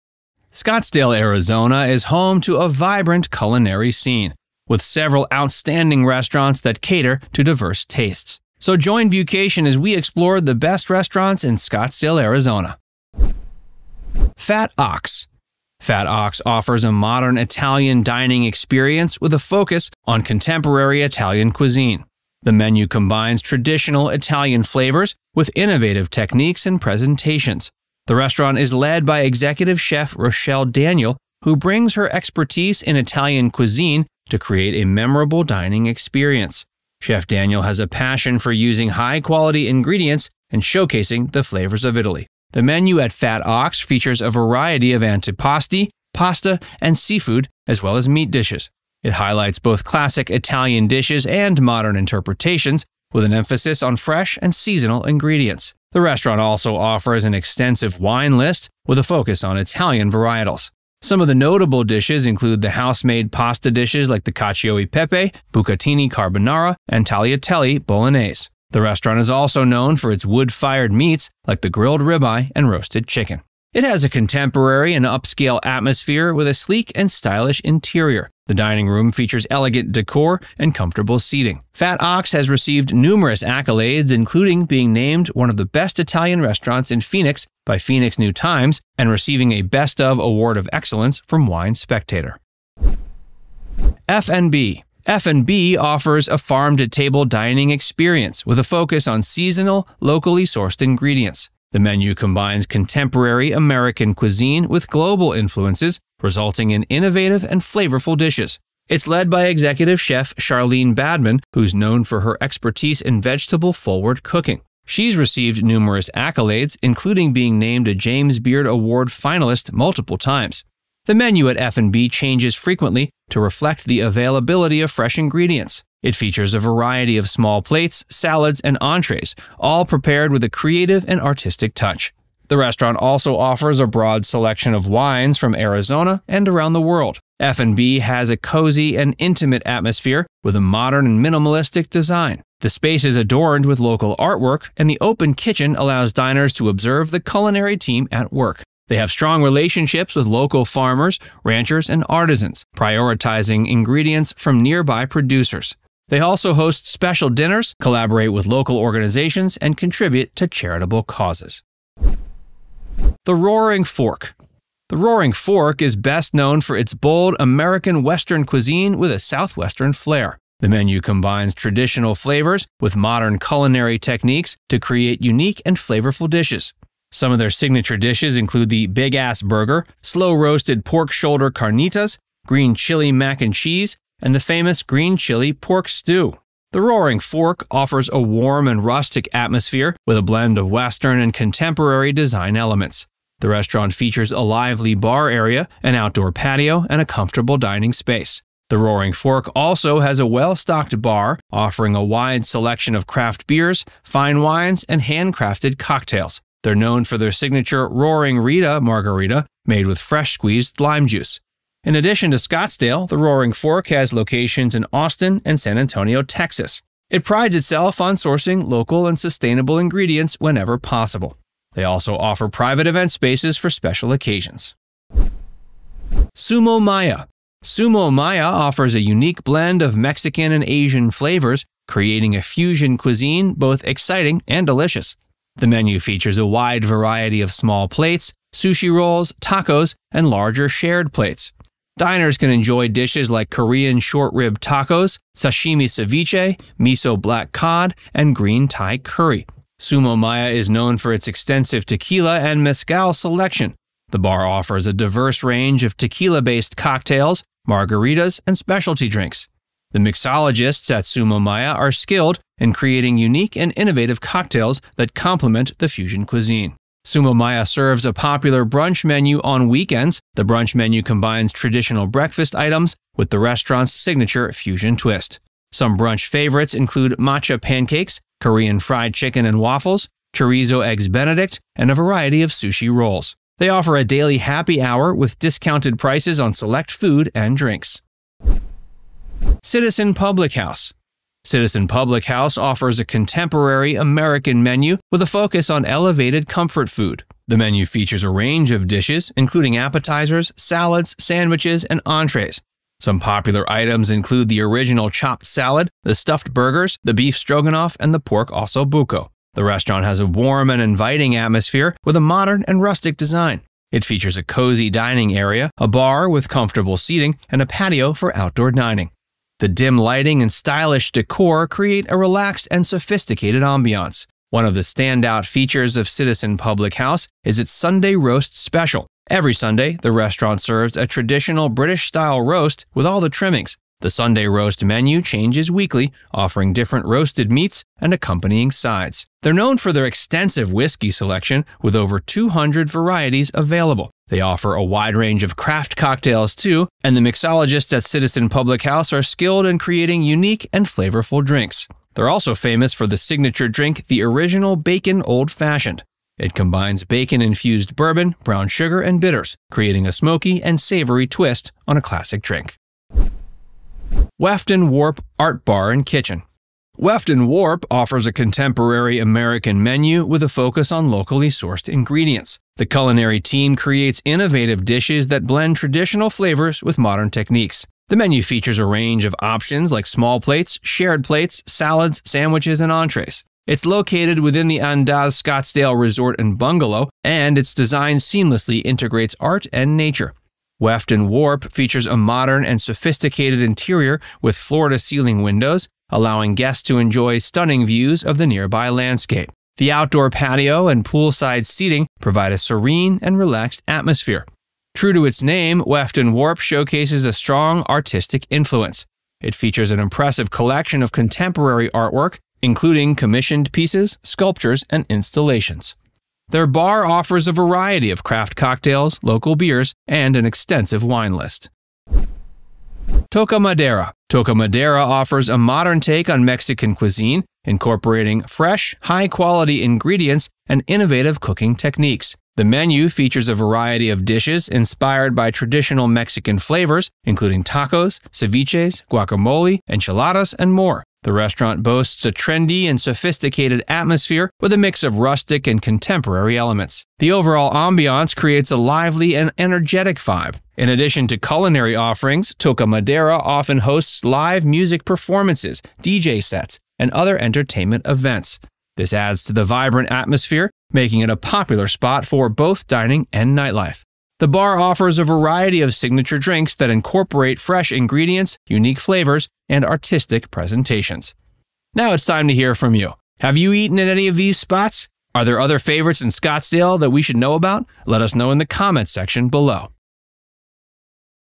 Best Restaurants in Scottsdale, Arizona
Scottsdale, Arizona, is home to a vibrant culinary scene, with several outstanding restaurants that cater to diverse tastes. Among the top establishments in the city are Fat Ox, SumoMaya, Weft + Warp, Postino, and many more!

00:00 - Intro
00:14 - Fat Ox
01:33 - FnB
02:43 - The Roaring Fork
03:46 - SumoMaya
04:49 - Citizen Public House
06:04 - Weft & Warp Art Bar + Kitchen
07:03 - Toca Madera
07:52 - Outro

Fat Ox is a renowned restaurant that offers a modern Italian dining experience. Executive Chef Rochelle Daniel combines traditional Italian flavors with innovative techniques to create a unique and memorable menu. From the house-made pastas like Cacio e Pepe and Bucatini Carbonara to the wood-fired meats like the Grilled Ribeye, Fat Ox delights diners with its contemporary Italian cuisine. The stylish and sophisticated ambiance makes it a popular spot for special occasions or intimate gatherings.

SumoMaya is another standout restaurant in Scottsdale, known for its fusion of Mexican and Asian flavors. The menu features a wide array of creative dishes, such as the Ahi Tuna Tostada, Korean Short Rib Tacos, and Miso-Marinated Black Cod. The vibrant atmosphere, modern decor, and lively bar scene make SumoMaya a go-to destination for a memorable dining experience.

Weft + Warp, located at the Andaz Scottsdale Resort & Bungalows, offers a unique dining experience with its modern American cuisine. The restaurant embraces the region's culinary traditions, showcasing locally sourced ingredients in dishes like the Sonoran Pasta and Grilled Arizona Striped Bass. The stunning desert views, contemporary design, and outdoor patio seating add to the charm of Weft + Warp.

Postino is a popular wine café that provides a relaxed and welcoming atmosphere. Known for its extensive wine list, Postino offers a variety of bruschetta and shareable plates that pair perfectly with their selection of wines. Whether it's the Brie and Apple with Fig Spread bruschetta or the Butcher's Block charcuterie board, Postino guarantees a delightful experience for wine enthusiasts and food lovers alike. Join ViewCation as we explore the best restaurants in Scottsdale, Arizona.